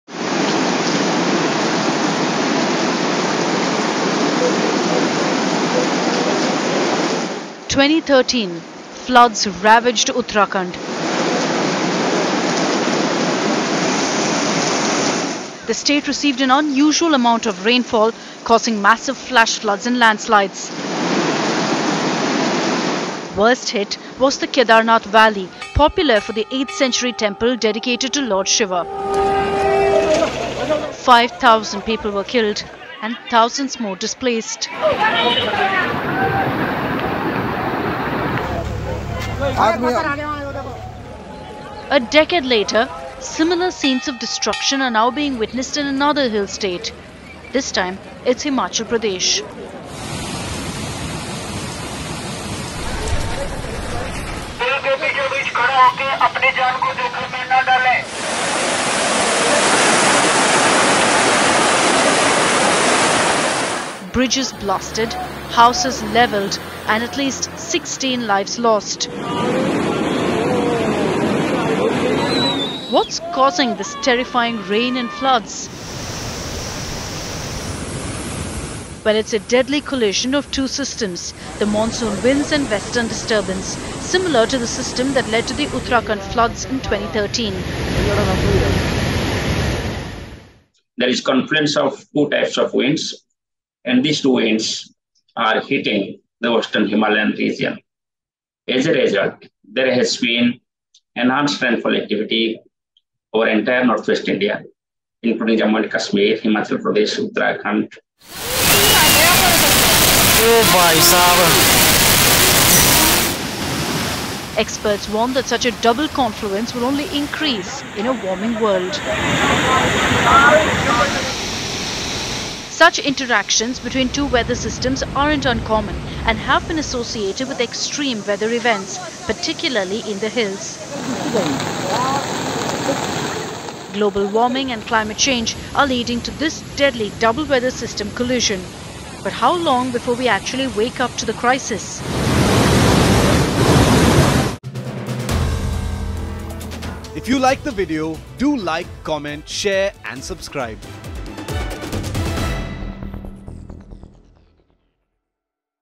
Devastating Floods Engulf Mountain State, 2013 Uttarakhand Deluge Re-run In 2023?
Images coming from Himachal Pradesh are reminding people of the horrific floods that devastated the state of Uttarakhand in 2013. 10 years later, a similar Himalayan tragedy is now unfolding in the state of Himachal Pradesh. what is causing this unprecedented rainfall?